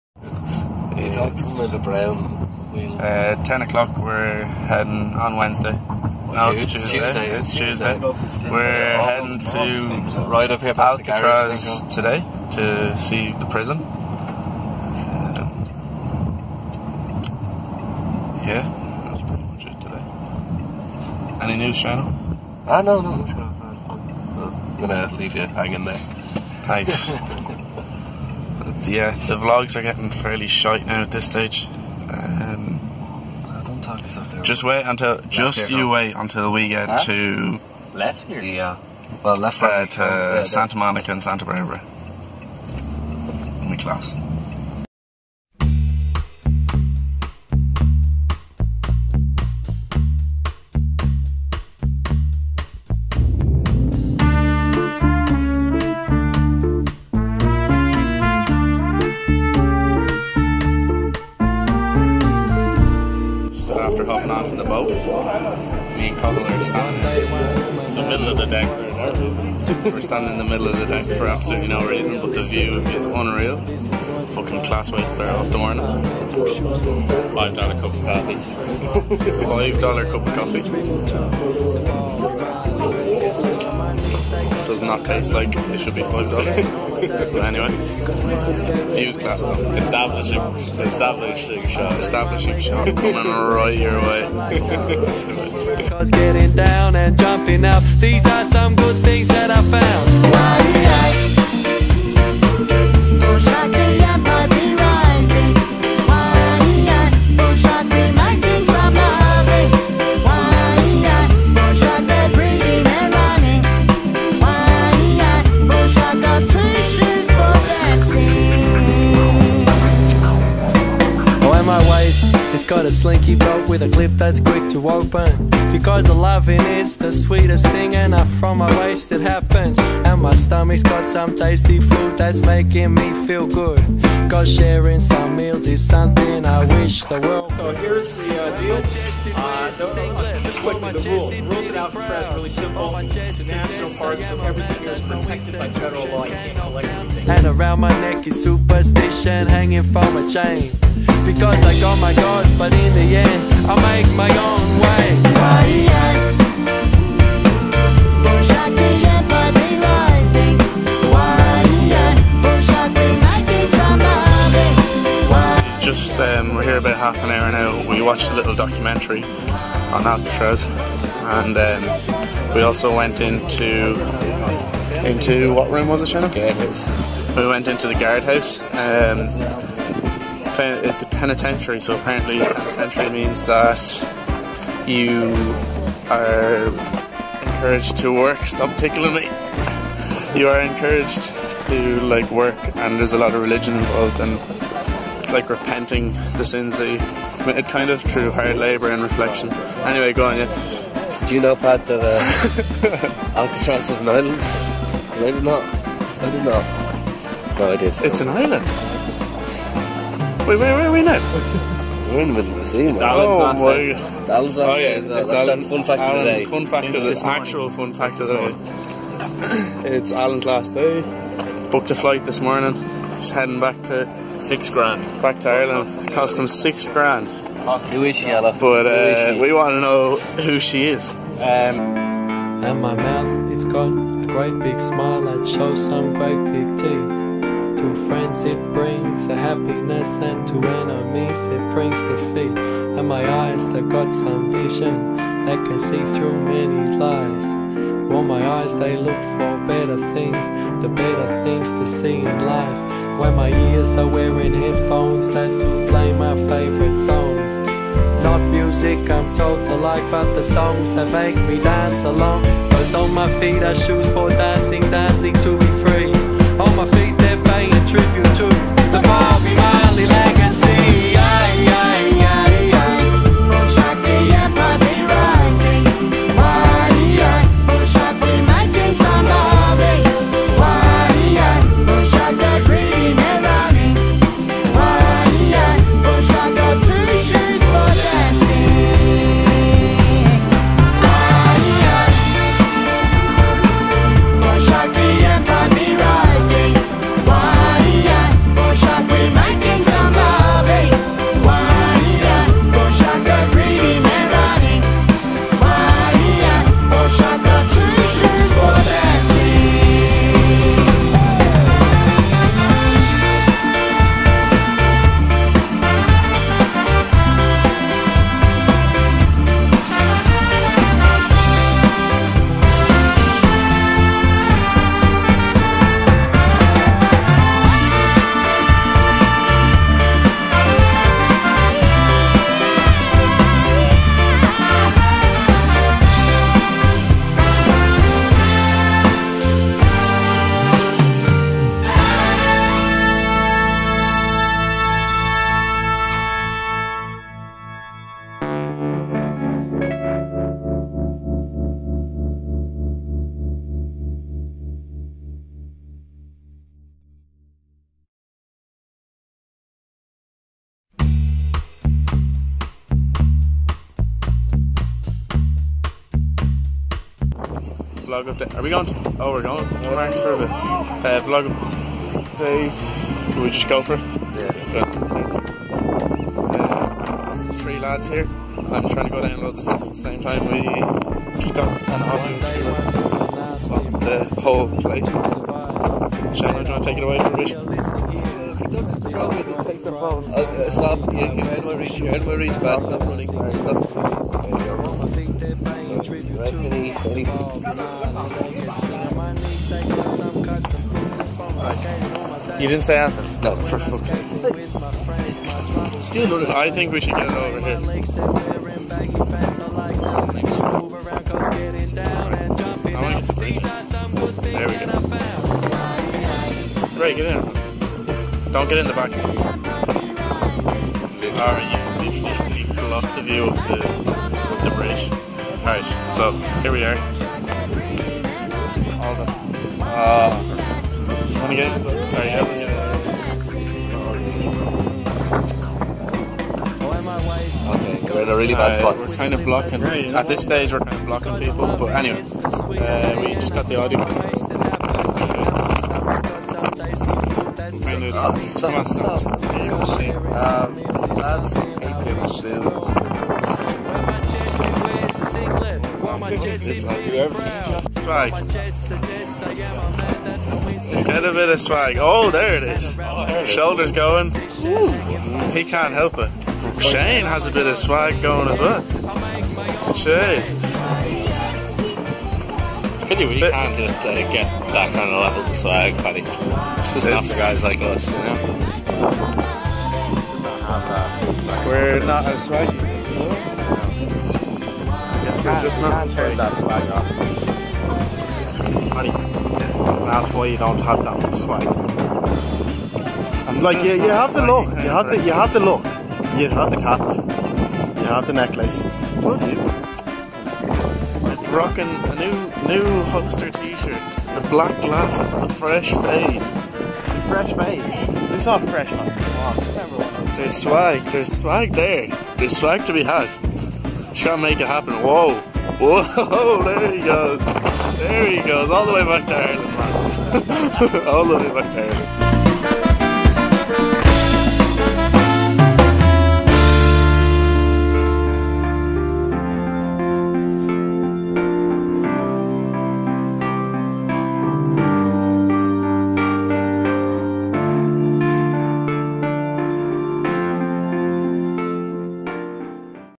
San Francisco Day 18 (Vlog 26)
We visited Alcatraz penitentiary today. It was a very interesting day out. Hope you enjoy!!